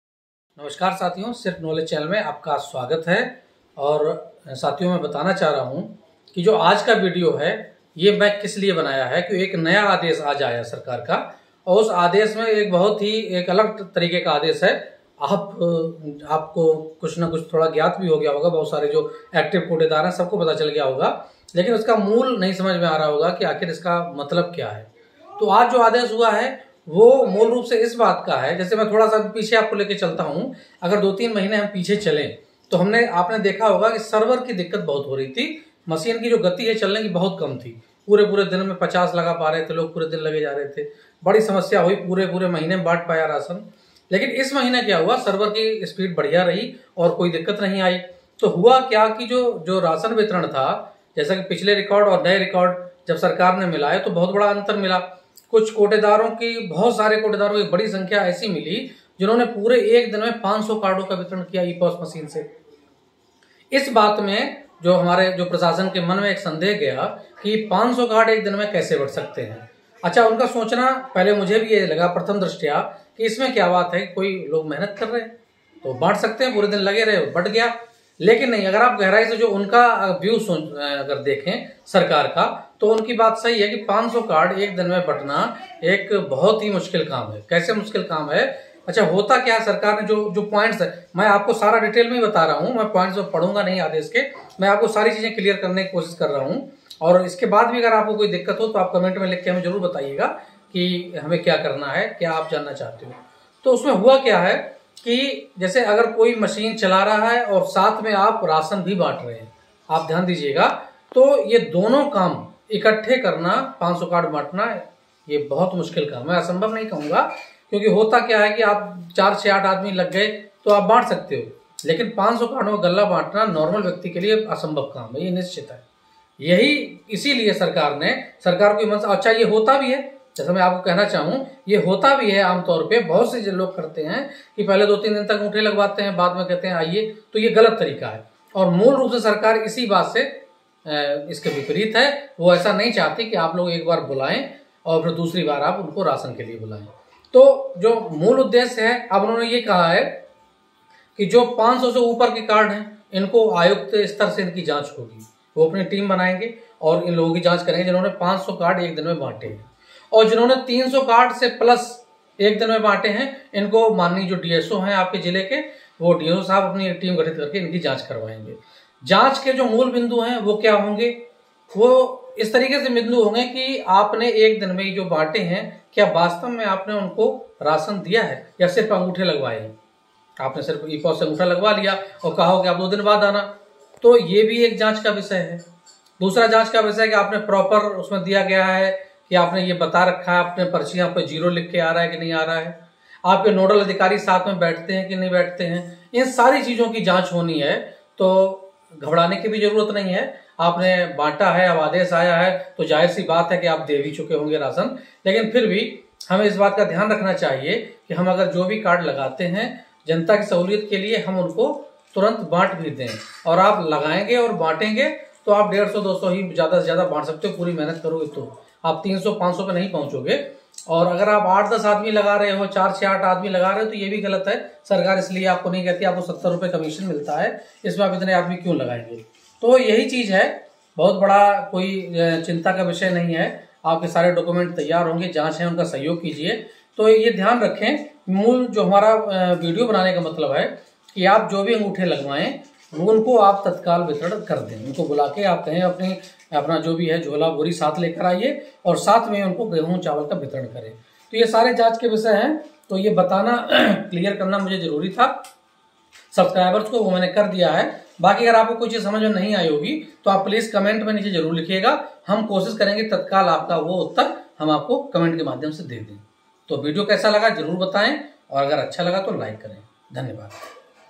कोटेदारों पर आई जांच की मुसीबत तीन सौ कार्ड वाले कोटेदारों की होगी जांच
कोटेदारों पर आई जांच की मुसीबत 300 कार्ड वाली कोटेदारों की होगी जांच